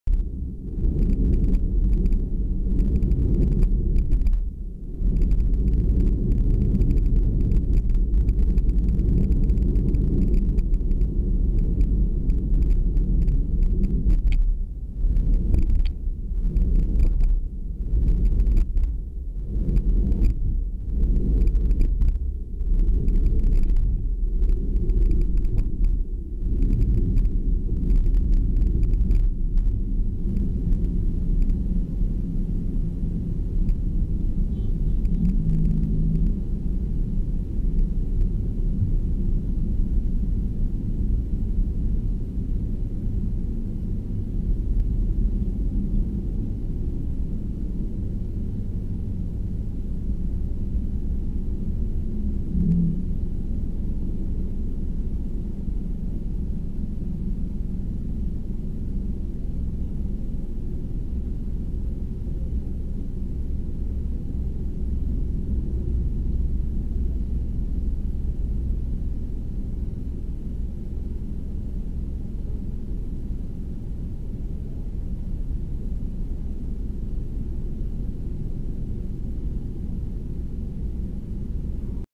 Cerrarán elevados de la avenida Expreso V Centenario
El Ministerio de Obras Públicas  cerrará este martes el elevado Expreso V Centenario con avenida Duarte, en horario de 10:30 de la noche a 5:00 de la madrugada del día de mañana, el cierre se debe a que estarán realizando mantenimiento general.
También, la institución gubernamental cerrará el acceso al elevado del  Expreso quinto Centenario con calle Hermanos Pinzón para realizar trabajos que concluirán este miércoles.
Brigadas del Obras Públicas estarán en estas estructuras realizando labores de recogida de desechos sólidos, barrido y recogida de agregados, control de maleza, limpieza general, supervisión eléctrica y estructural, y lavado de revestimiento, informó el ministerio.